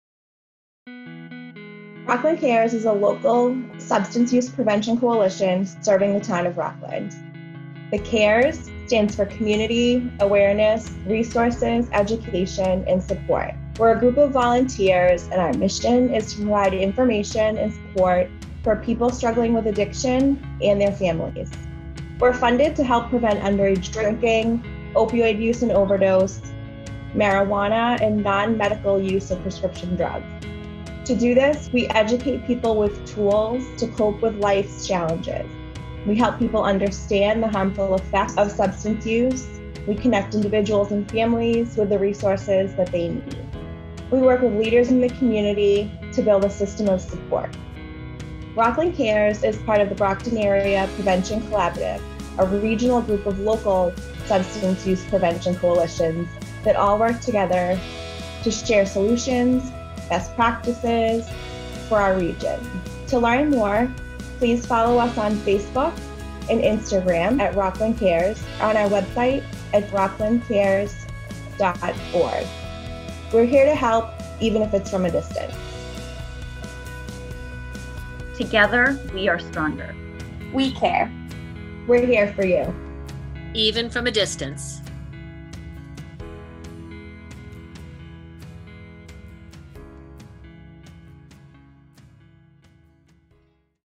Rockland CARES PSA 2020
Rockland CARES is the local substance use prevention coalition serving the Town of Rockland.

Rockland CARES is here for you - even from a distance - during this time of social distancing.